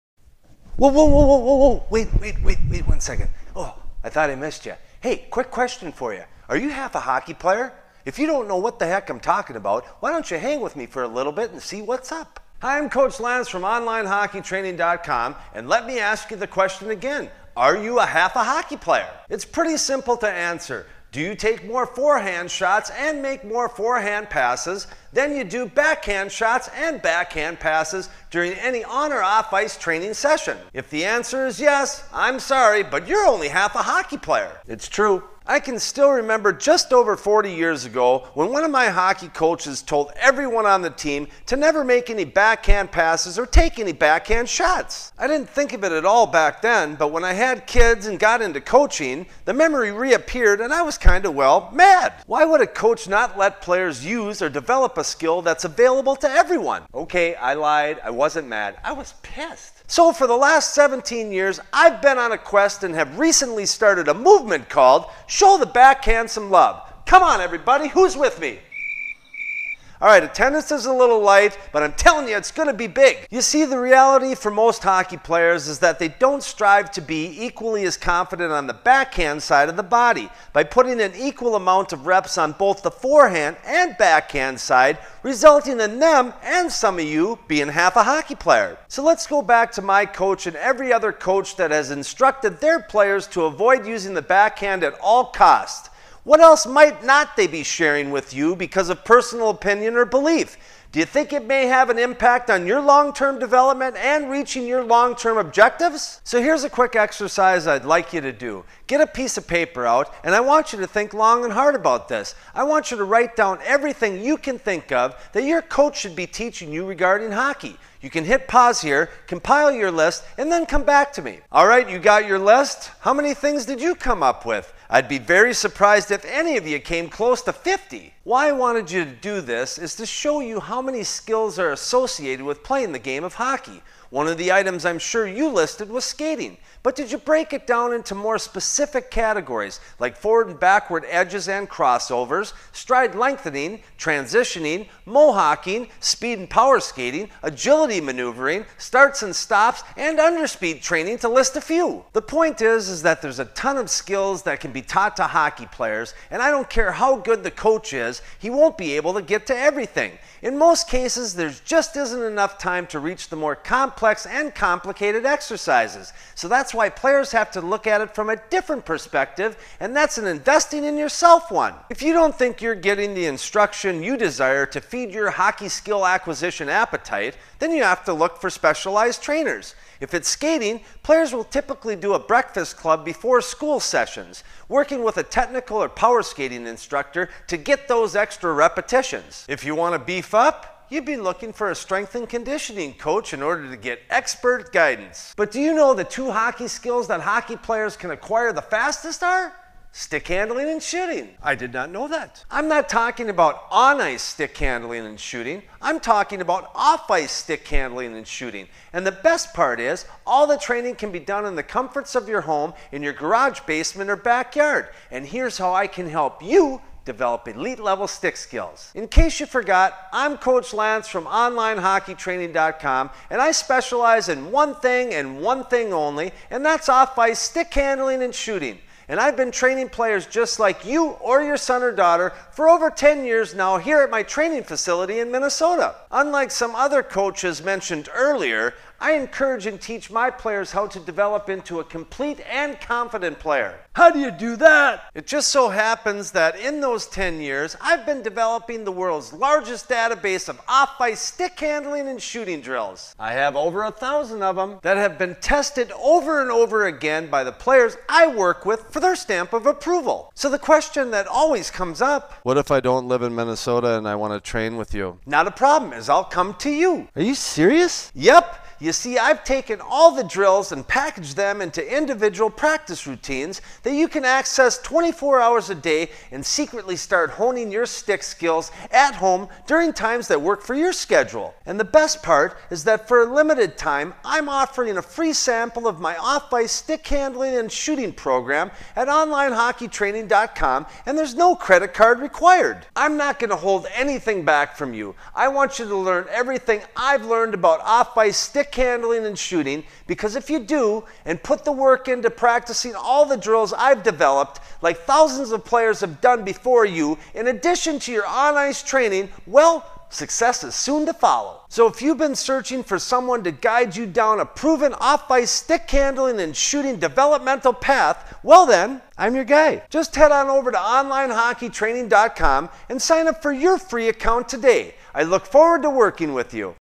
Are you half a hockey player? Online Hockey Training is the source for your off ice hockey training.
With the forehand and backhand in your arsenal are you taking full advantage of the backhand?